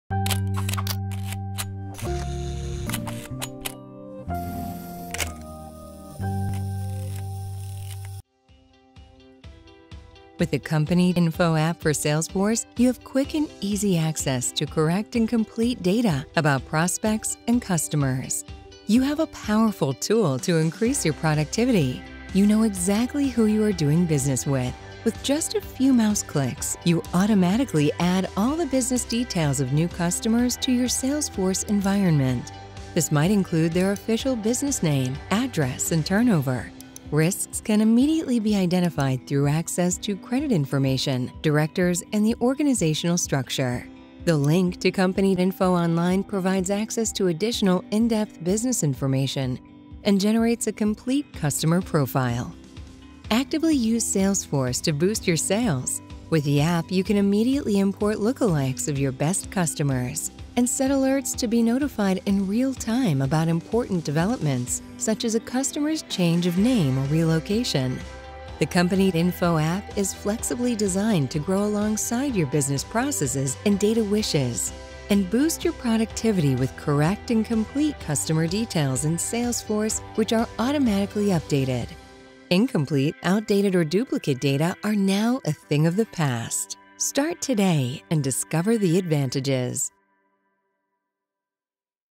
Company.info App for Salesforce
With the Company.info App for Salesforce, you have quick and easy access to correct and complete data about prospects and customers. You can automatically add all the business details of new customers to your Salesforce environment. Risks can immediately be identified through access to credit information, directors and the organisational structure. Boost your productivity with correct and complete customer details in Salesforce which are automatically updated.